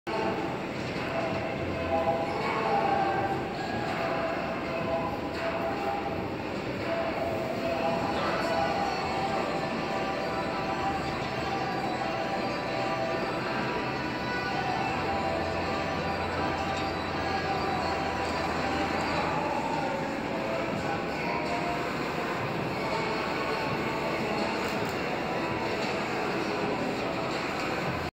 LED Balloon Darts filled with Neon Paint - Great for College Events or University Sponsored Events
LED Balloon Darts is the latest and greatest attraction As expected, it's a big hit at colleges across the midwest for university sponsored events. We provide the portable wall, carpeting, filled balloons with paint, darts, cleaning supplies, as well as extra balloons and paint depending on the duration of the event and event attendees. We're the largest college game and entertainment supplier in the midwest!